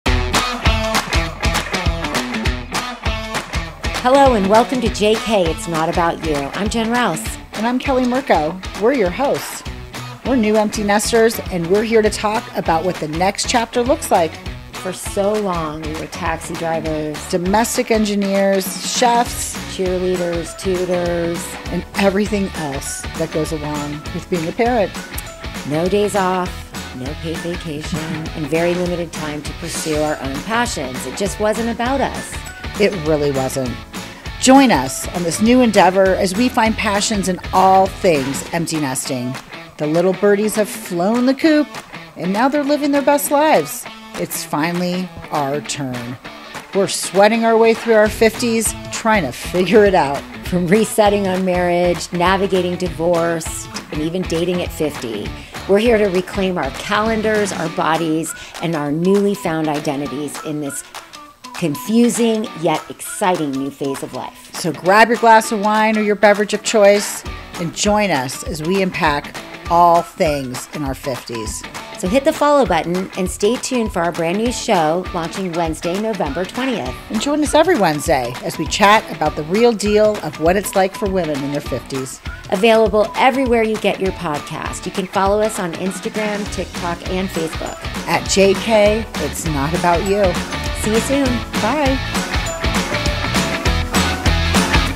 JK It's Not About You Trailer YouTube
Hello and welcome to JK It's Not About You, I’m Jen Rouse and I’m Kelli Murcko, and we’re your hosts. We’re new empty nesters, and we’re here to talk about what the next chapter looks like. For so long, we were taxi drivers, domestic engineers, chefs, tutors, cheerleaders and everything else that goes along with being a parent! No days off, no paid vacation, and very limited time to pursue our own passions.  It just wasn’t about us… Join us on this new endeavor as we find passions in all things empty nesting. The little birdies have flown the coup and living their best lives, and now, it’s finally our turn. We’re sweating through our 50’s trying to figure it out. From resetting on marriage, navigating divorce, dating at 50, we're here to reclaim our calendars, our bodies, and our newly found identities in this confusing yet exciting new phase of life. So grab your glass of wine or your beverage of choice and your cozy sweats and join us as we unpack all things real in our 50’s. Nov. 20 and join us every Wednesday as we chat about the real deal of what life is like for women in their 50’s, available everywhere you get your podcast.